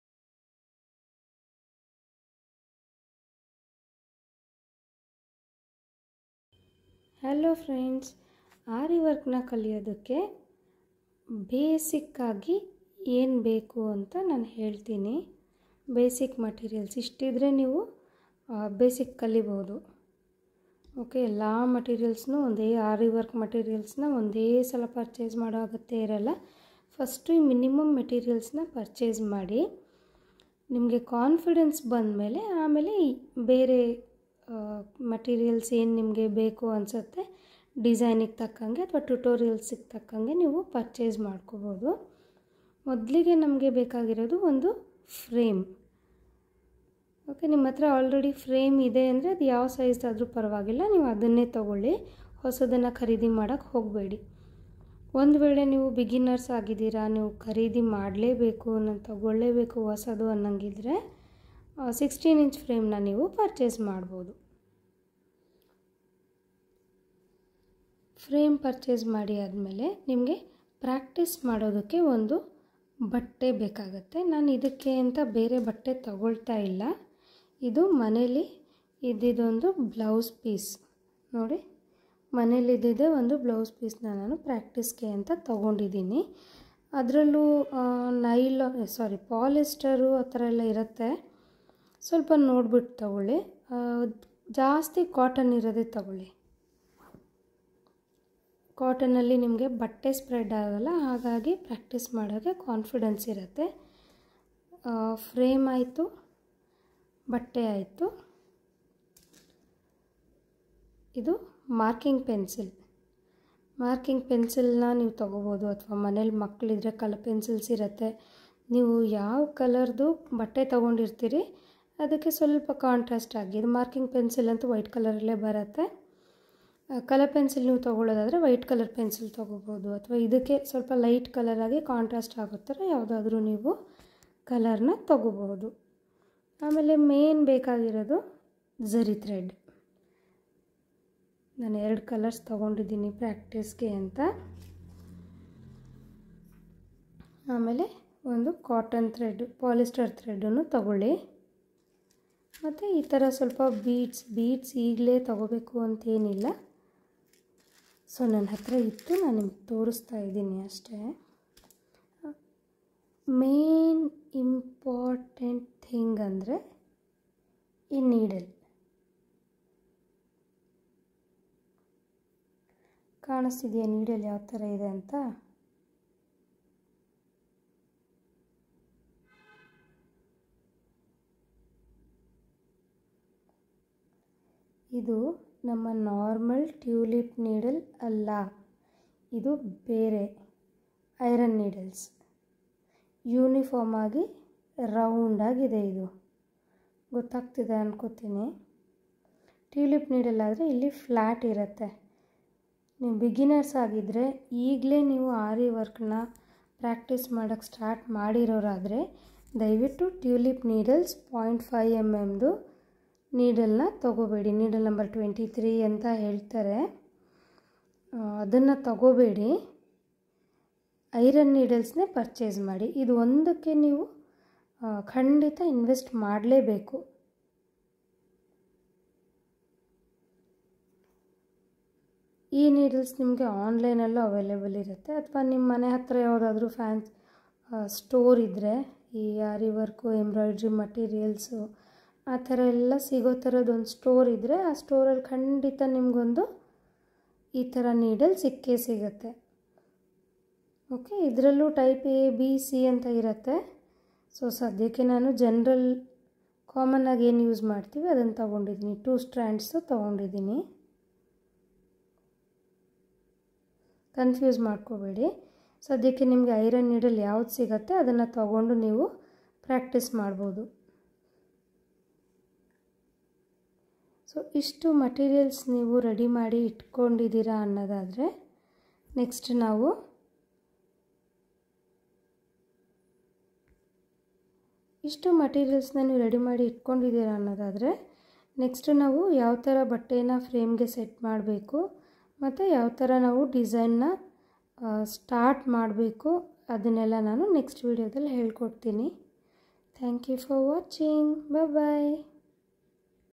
Aari work day#1 ||Basic Materials required
Aari work basic materials which are enough to start off the aari learning ...